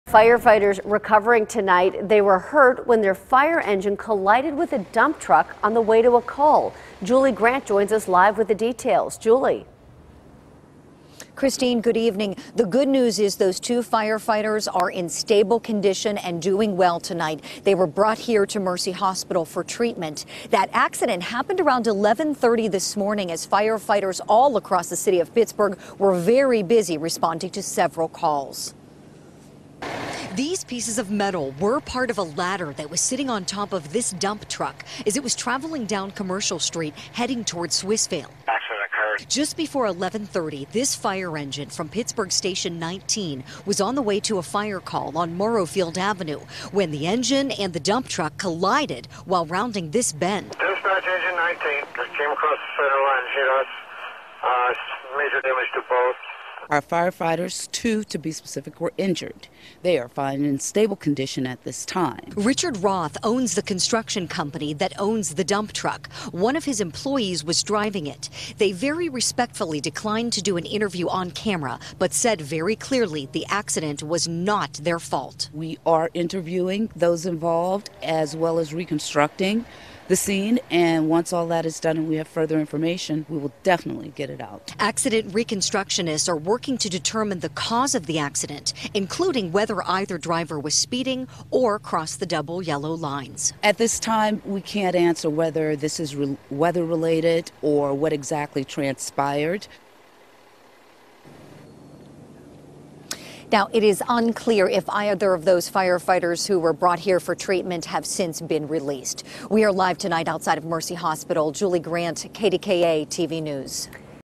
2 Firefighters Injured In Squirrel Hill Crash
Two firefighters were injured when their firetruck collided with a dump truck in Squirrel Hill Thursday morning.